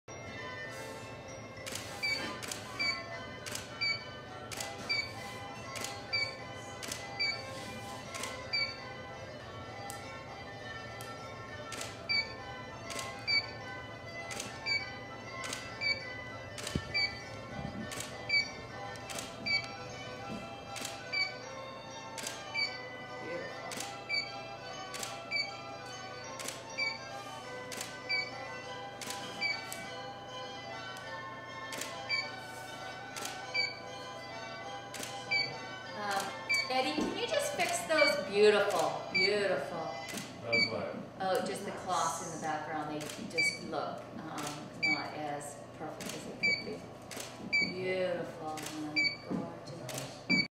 Kate Mansi on the set of Better Living
A little up-close and personal preview of Kate Mansi working out for the BETTER LIVING book photo shoot at her home.